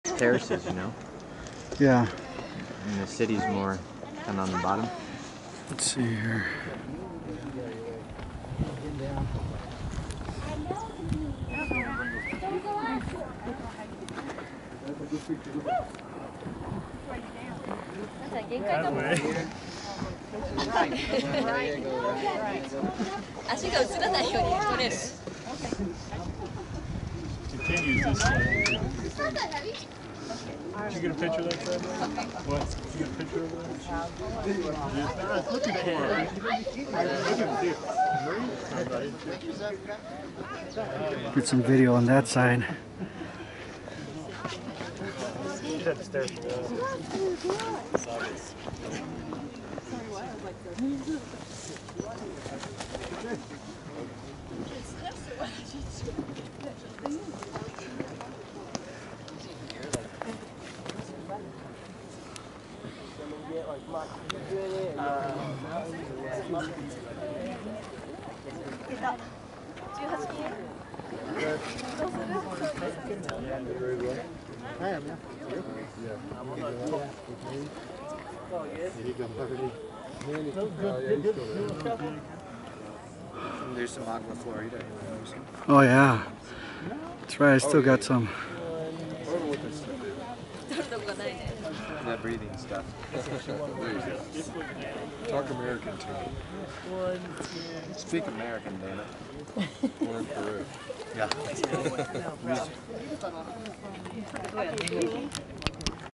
Huaynapichu view of Machu Pichu
A short but steep ascent to the top of Huaynapichu mountain affords you a great areal view of Machu Pichu where you can see how the city is laid out in the shape of the condor.  The view from Huaynapichu shows the Condor upside down.